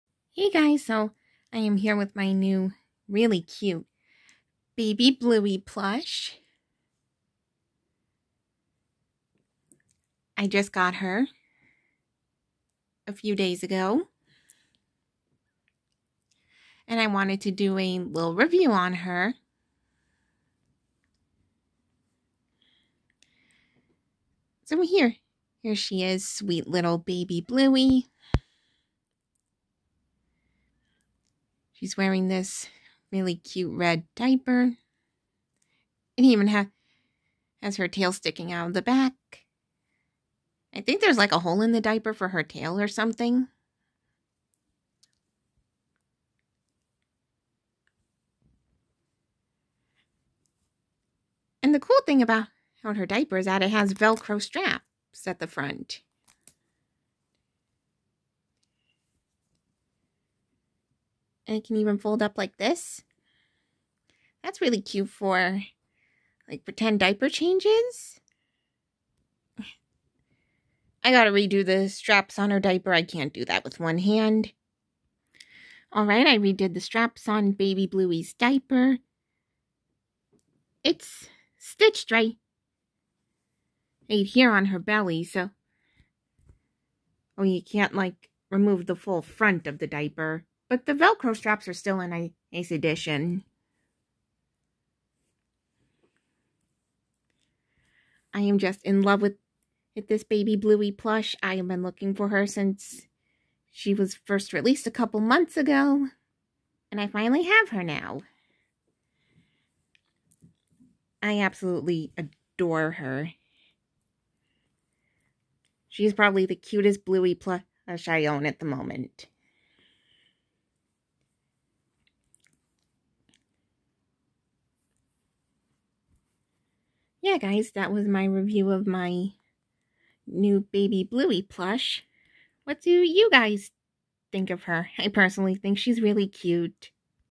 Baby Bluey Plush Review
I decided to do a review of my new Baby Bluey plush. What do you think of her?